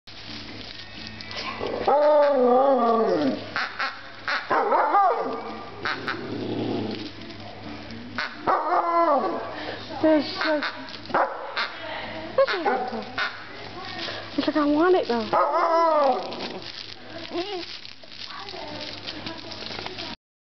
Malamute puppy wants
Hunter (belongs to my friend) really wanted his new toy and had to tell everyone about it till he got it, it was so adorable I just had to film it.